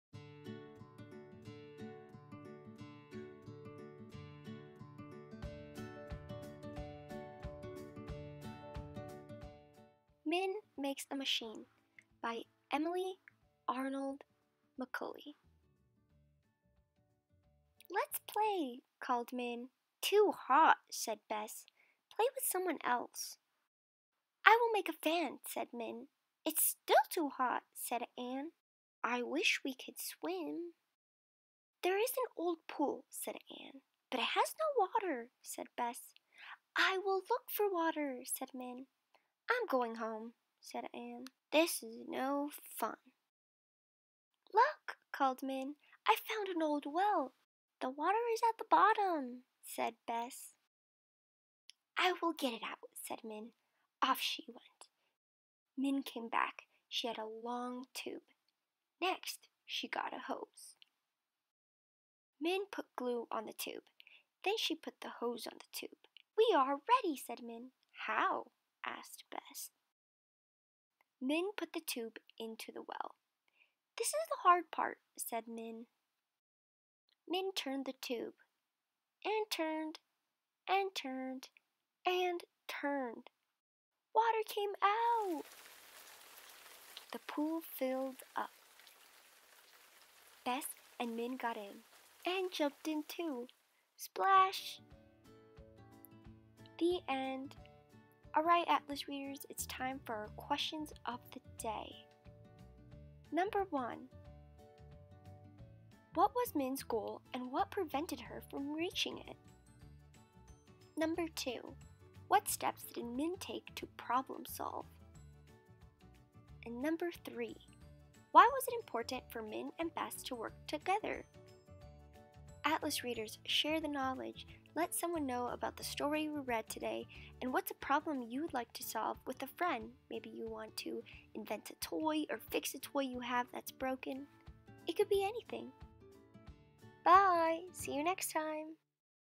Min makes a Machine- Read Aloud: Toys and Play
Welcome to our fourth book in our Toys & Play Module Series! Today we are reading Min makes a Machine by Emily Arnold McCully and Illustrated by David Roberts. This book is about a spunky elephant named Min who is determined to create a solution for her and her friends to cool of on this extremely hot day! Min shows us that we can all be problem solvers if we put our minds to it!

 Head to our playlist tab and click on the Toys & Play Module Read Aloud Series playlist to read more books about curiosity, invention, and dreams!